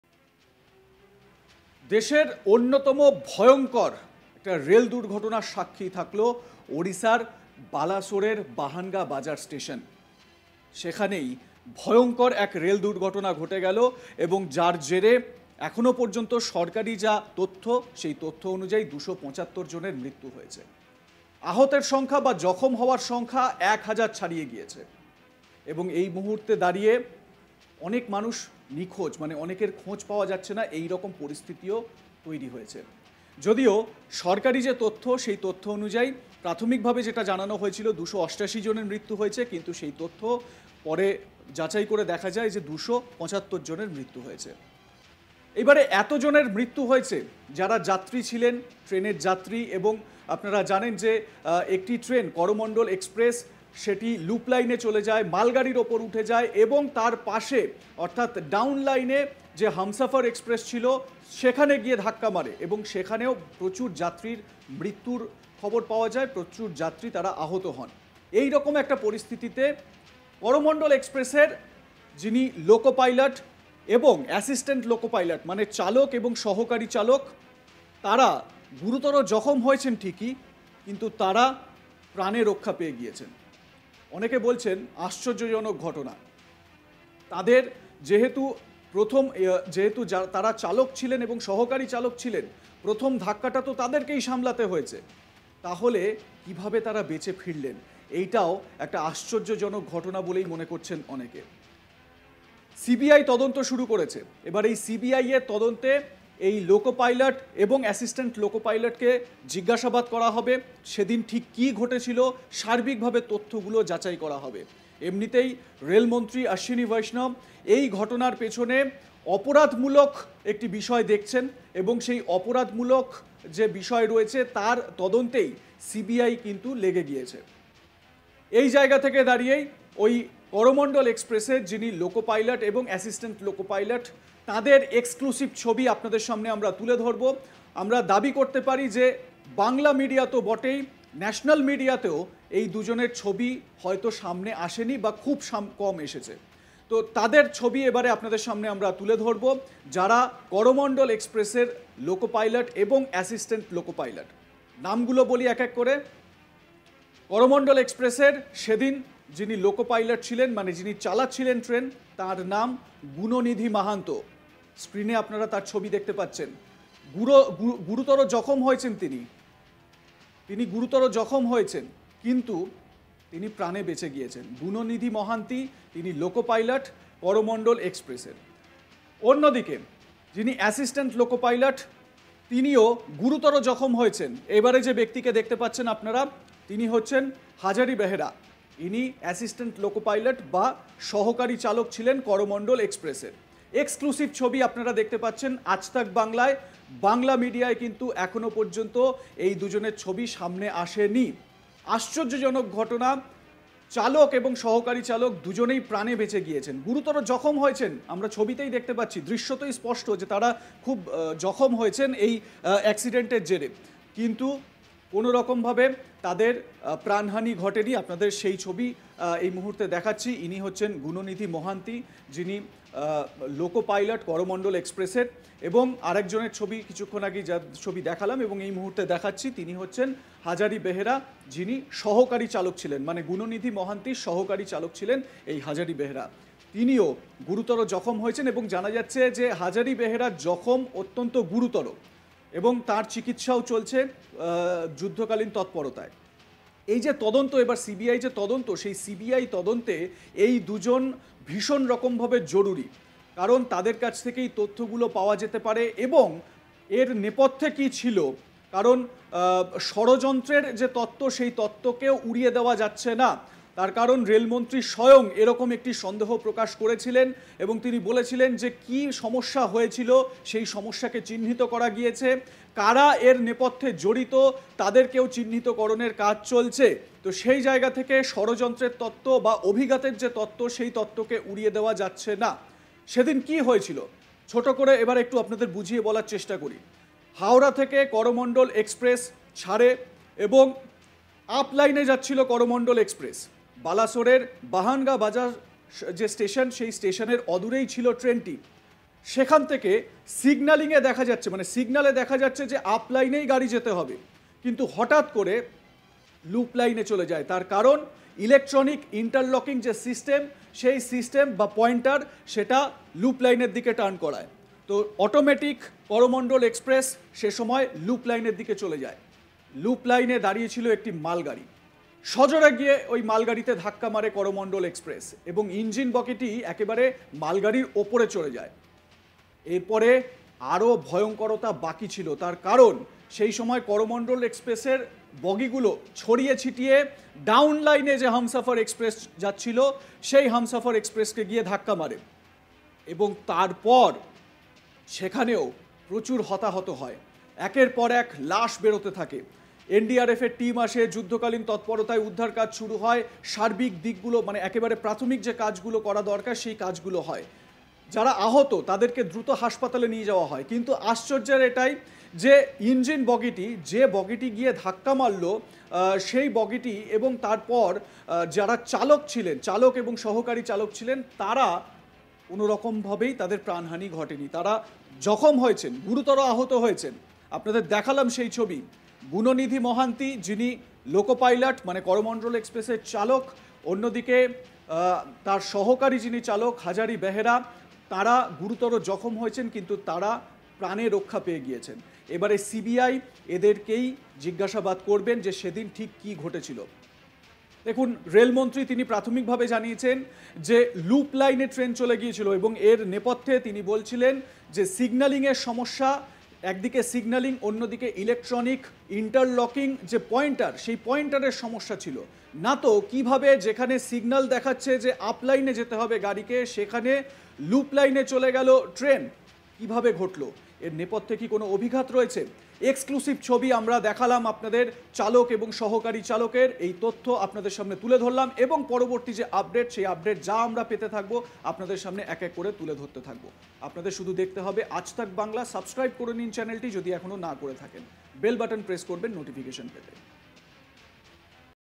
অভিশপ্ত করমণ্ডলের চালক ও সহকারী চালকের ছবি আজতক বাংলায় | Coromandel Express Accident | Train Driver
Aaj Tak Bangla Deputy Editor Indrajit Kundu's EXCLUSIVE Report: Coromandel Express Loco Pilot's Image only on Aaj Tak Bangla

অভিশপ্ত করমণ্ডলের চালক এবং সহকারী চালকের ছবি আজতক বাংলায় | Coromandel Express Accident | Loco Pilot